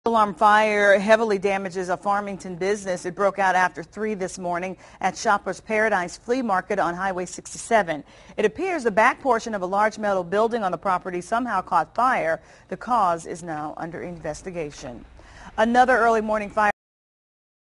Fire Destroys Part Of Flea Market Near Farmington
A massive two alarm fire breaks out at the "Shopper Paradise" flea market between Farmington and Park Hills Missouri early Tuesday morning. The broke out around 1:30 a.m., destroying the back portion of the metal structure located on Highway 67.&nbsp; No injuries were reported.